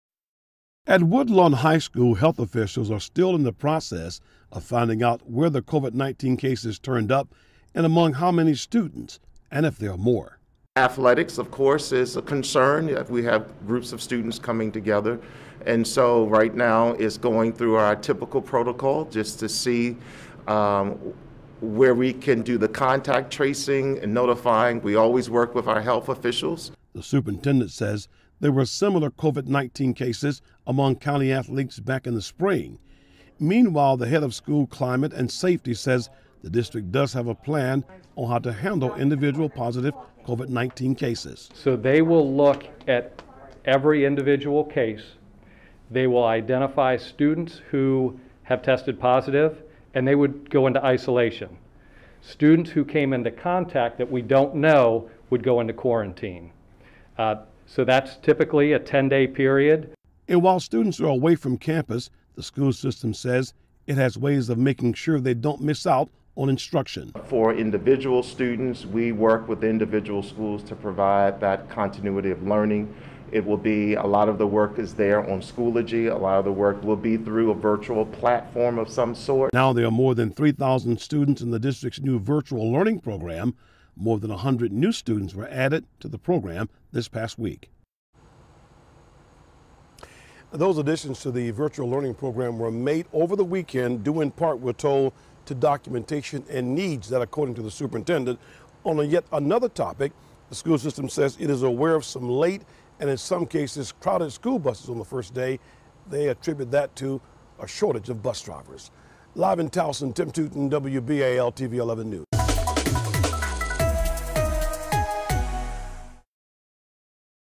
BCPS officials explain COVID-19 plans amid positive cases
Just two days into the school year and one Baltimore County school is dealing with positive COVID-19 cases among its student athletes.